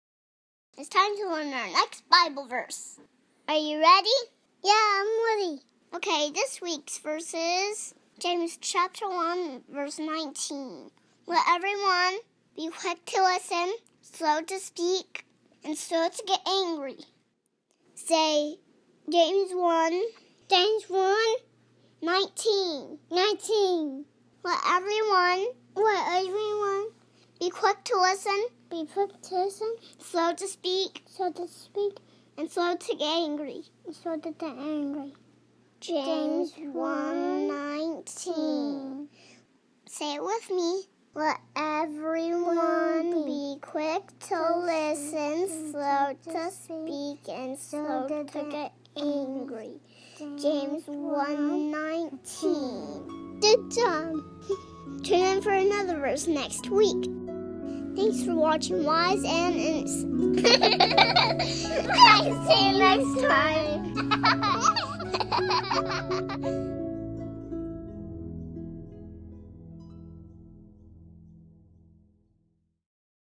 JAMES 1:19 Wise and Innocent
It's time to learn our next Bible verse! James 1:19 "Let everyone be quick to listen, slow to speak, and slow to get angry."